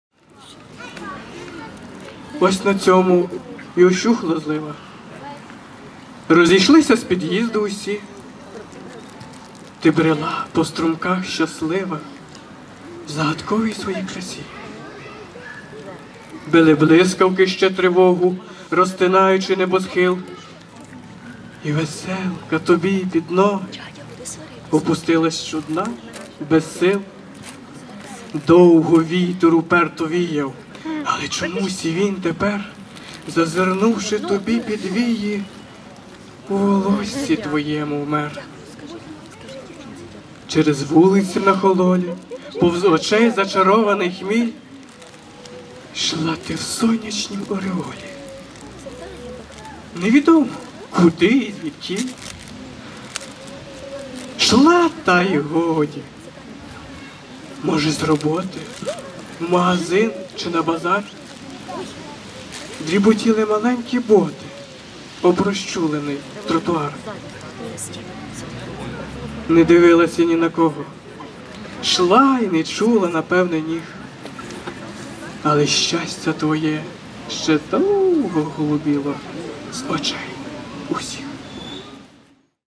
Вірш Закохана
Мистецький квартирник на підтримку ЗСУ
Хотин
27 травня 2022 рік

Організатор - ЗА театр юного глядача Хотинського БНТД
За підтримки Агенції розвитку міста
З допомогою Хотинської міської публічної бібліотеки
Хотинської дитячої художньої школи
а також хотинських художниць Ольги Дідик (Микита), Зої Піхут, Валентини Іванової та майстрині Тетяни Дашук.